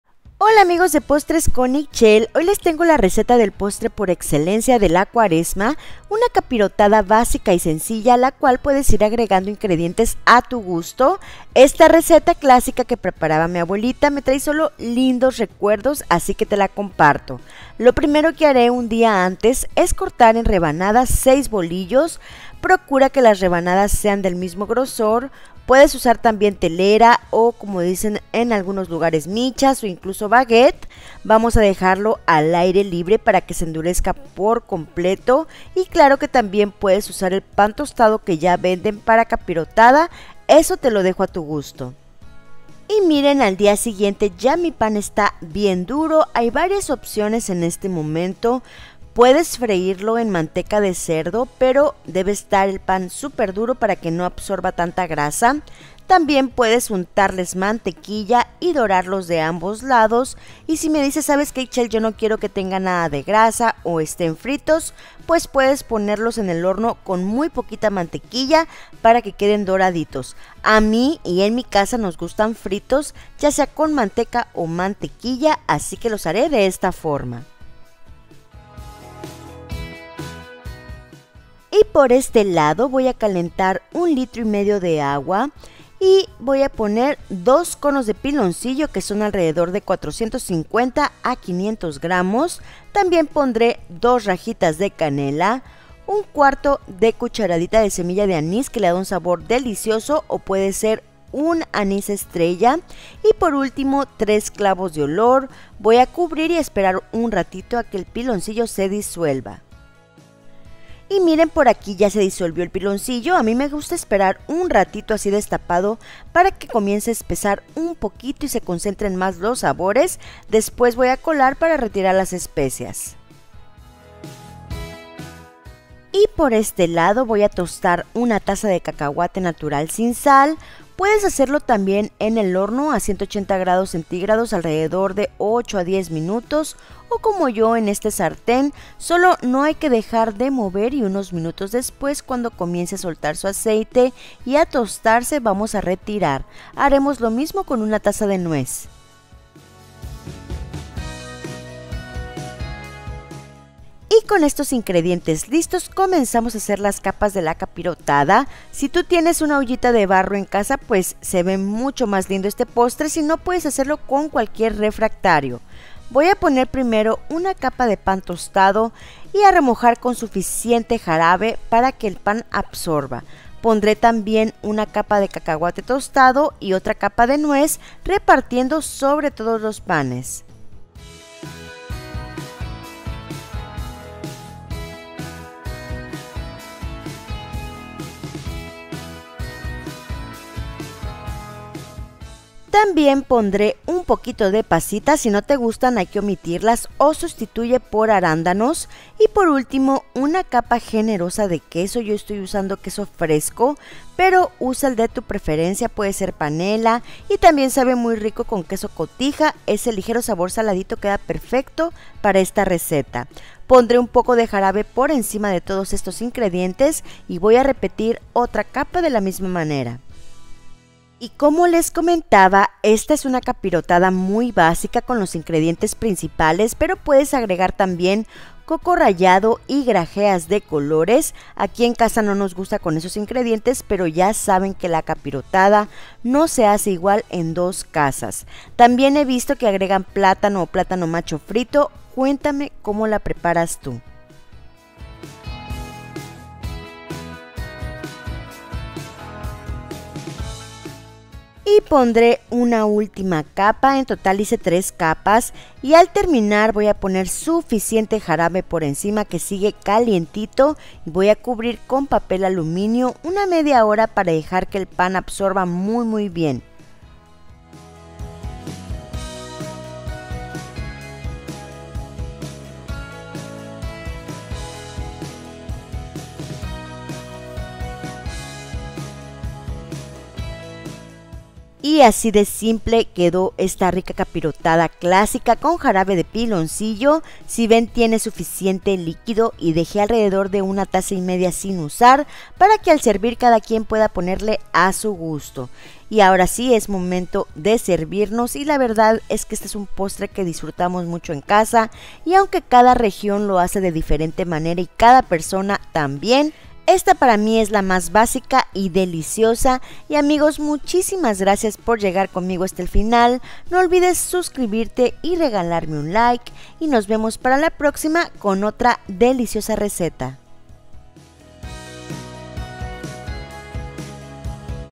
CAPIROTADA TRADICIONAL CON JARABE DE PILONCILLO | POSTRES CON IXCHEL
Hola amigos de @postresconIxchel hoy les tengo la receta de una capirotada clásica y muy básica para que puedas agregar los ingredientes que más te gusten, un postre mexicano típico de diferentes regiones que es muy fácil de hacer y simplemente delicioso ¡vamos a comenzar y no olvides suscribirte a mi segundo canal @encasaconixchel 💗

RECETA:
- 6 bolillos cortados en rebanadas
- 1 taza de cacahuate tostado
- 1 taza de nuez tostada y picada
- 1 taza de pasitas
- 250 gramos de queso fresco o panela

Jarabe de piloncillo
- 1.5 litros de agua
- 2 conos de piloncillo (450 a 500 gramos)
- 2 rajas de canela
- 1/4 cucharadita de anís
- 3 clavos de olor